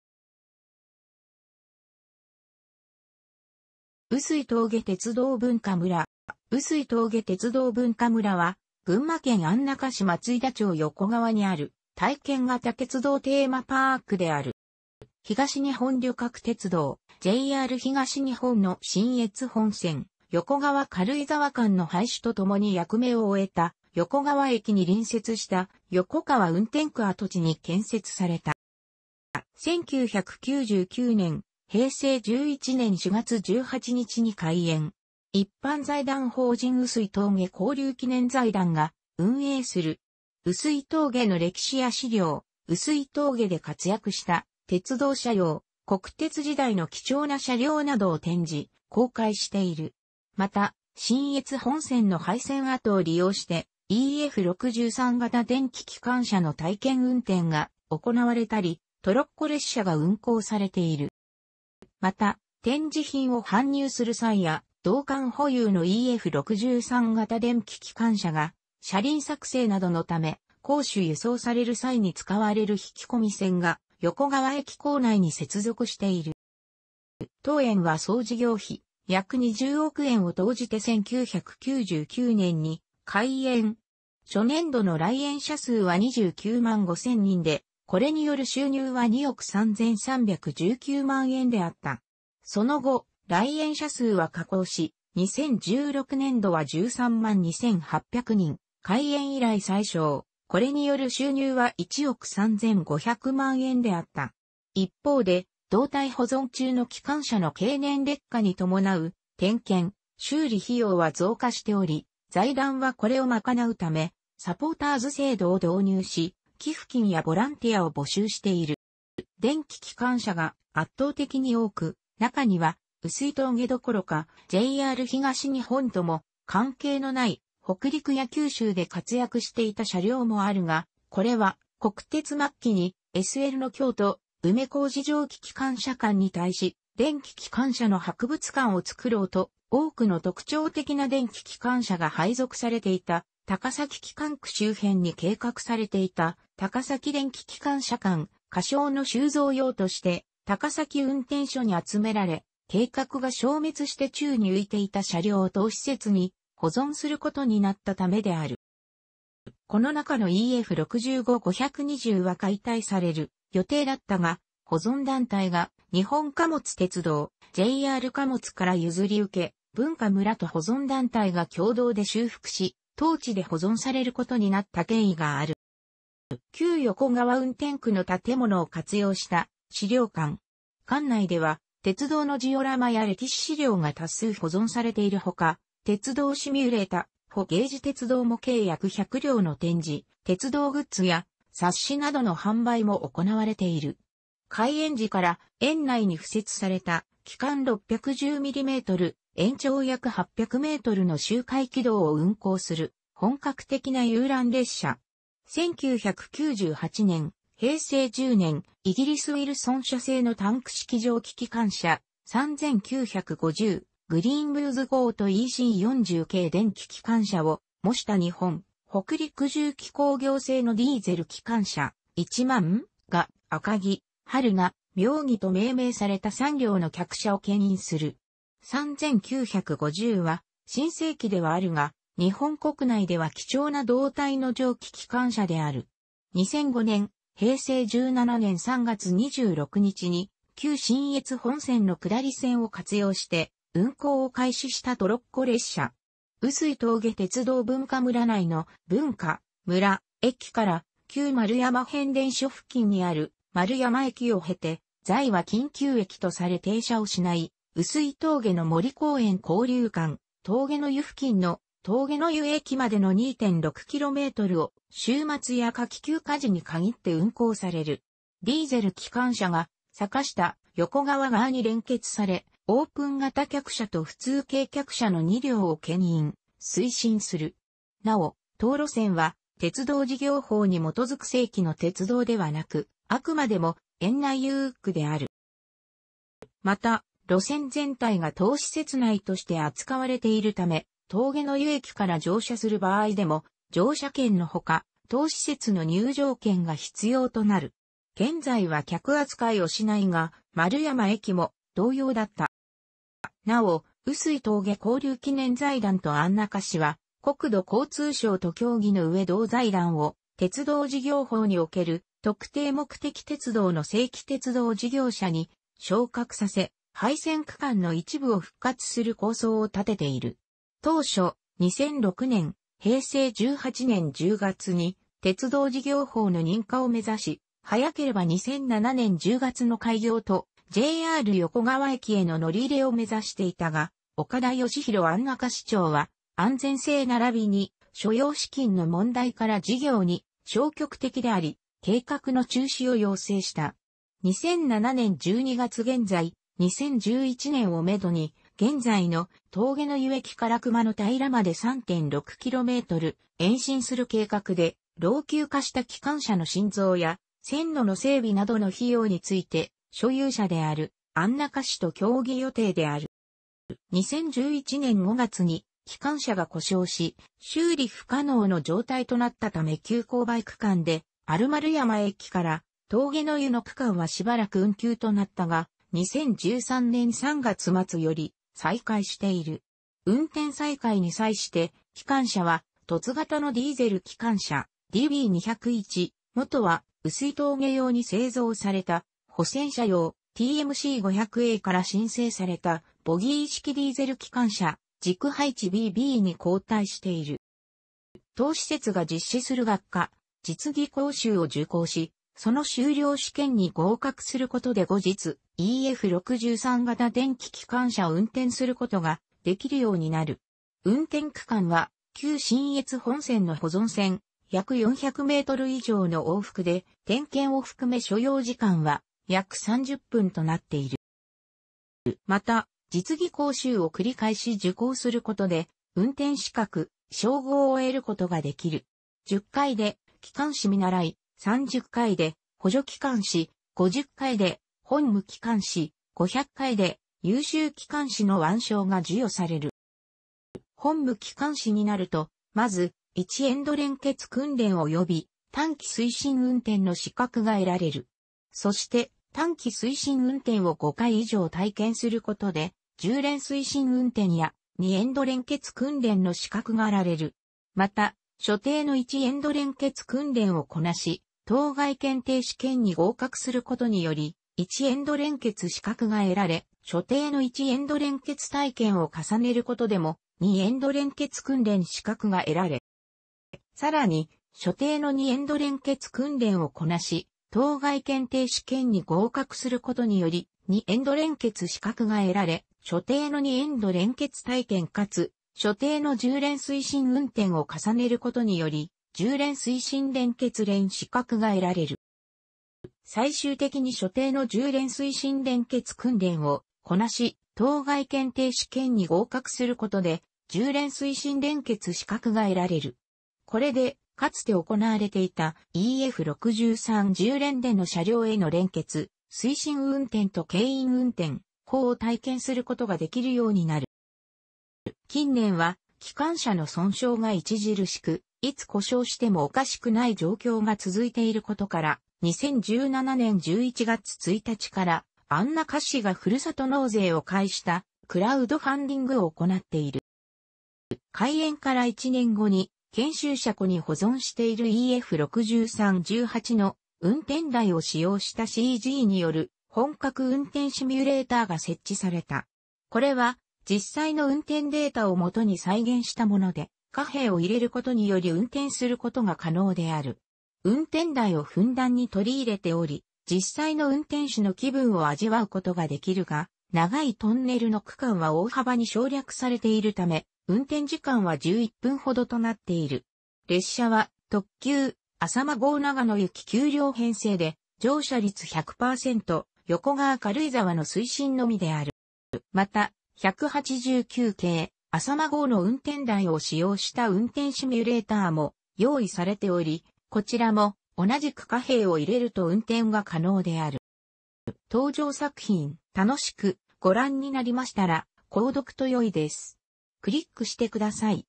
碓氷峠鉄道文化むら
碓氷峠鉄道文化むら（うすいとうげてつどうぶんかむら）は、群馬県安中市松井田町横川にある体験型鉄道テーマパークである。

東日本旅客鉄道（JR東日本）の信越本線横川 - 軽井沢間の廃止とともに役目を終えた、横川駅に隣接した横川運転区跡地に建設された。

1999年（平成11年）4月18日に開園。一般財団法人碓氷峠交流記念財団が運営する。碓氷峠の歴史や資料、碓氷峠で活躍した鉄道車両、国鉄時代の貴重な車両などを展示・公開している。また、信越本線の廃線跡を利用してEF63形電気機関車の体験運転が行われたり、トロッコ列車が運行されている。

また、展示品を搬入する際や、同館保有のEF63形電気機関車が車輪削正などのため、甲種輸送される際に使われる引き込み線が、横川駅構内に接続している。

当園は総事業費約20億円を投じて1999年に開園。初年度の来園者数は29万5000人で、これによる収入は2億3319万円であった。その後、来園者数は下降し、2016年度は13万2800人（開園以来最少）、これによる収入は1億3500万円であった。一方で、動態保存中の機関車の経年劣化に伴う点検・修理費用は増加しており、財団はこれを賄うため「サポーターズ制度」を導入し、寄付金やボランティアを募集している。

電気機関車が圧倒的に多く、中には碓氷峠どころかJR東日本とも関係のない、北陸や九州で活躍していた車両もあるが、これは国鉄末期に「SLの京都（梅小路蒸気機関車館）に対し、電気機関車の博物館を作ろう」と多くの特徴的な電気機関車が配属されていた高崎機関区周辺に計画されていた「高崎電気機関車館」（仮称）の収蔵用として高崎運転所に集められ、計画が消滅して宙に浮いていた車両を当施設に保存することになったためである。

この中のEF65 520は解体される予定だったが、保存団体が日本貨物鉄道（JR貨物）から譲り受け、文化むらと保存団体が協同で修復し、当地で保存されることになった経緯がある。

旧横川運転区の建物を活用した資料館。館内では、鉄道のジオラマや歴史資料が多数保存されている他、鉄道シミュレータ、HOゲージ鉄道模型約100両の展示、鉄道グッズや冊子などの販売も行われている。
開園時から、園内に敷設された軌間610mm、延長約800mの周回軌道を運行する本格的な遊覧列車。1998年（平成10年）、イギリス・ウィルソン社製のタンク式蒸気機関車（3950「グリーンブリーズ」号）とEC40形電気機関車を模した日本・北陸重機工業製のディーゼル機関車 (10000) が「赤城」「榛名」「妙義」と命名された3両の客車を牽引する。3950は新製機ではあるが、日本国内では貴重な動態の蒸気機関車である。
2005年（平成17年）3月26日に、旧信越本線の下り線を活用して運行を開始したトロッコ列車。碓氷峠鉄道文化むら内の「ぶんかむら」駅から、旧丸山変電所付近にある「まるやま」駅を経て（現在は緊急駅とされ停車をしない）、碓氷峠の森公園交流館「峠の湯」付近の「とうげのゆ」駅までの2.6kmを週末や夏期休暇時に限って運行される。ディーゼル機関車が坂下（横川）側に連結されオープン形客車と普通形客車の2両を牽引（推進）する。なお、当路線は鉄道事業法に基づく正規の鉄道ではなく、あくまでも園内遊具である。

また、路線全体が当施設内として扱われているため、とうげのゆ駅から乗車する場合でも、乗車券の他、当施設の入場券が必要となる。現在は客扱いをしないが、まるやま駅も同様だった。

なお、碓氷峠交流記念財団と安中市は、国土交通省と協議のうえ同財団を鉄道事業法における特定目的鉄道の正規鉄道事業者に昇格させ、廃線区間の一部を復活する構想を立てている。当初、2006年（平成18年）10月に鉄道事業法の認可を目指し、早ければ2007年10月の開業とJR横川駅への乗り入れを目指していたが、岡田義弘安中市長は、安全性ならびに所要資金の問題から事業に消極的であり、計画の中止を要請した。2007年12月現在、2011年をめどに、現在の「とうげのゆ駅」から熊ノ平まで3.6km延伸する計画で、老朽化した機関車の新造や線路の整備などの費用について、所有者である安中市と協議予定である。

2011年5月に機関車が故障し、修理不可能の状態となったため急勾配区間であるまるやま駅からとうげの湯の区間はしばらく運休となったが、...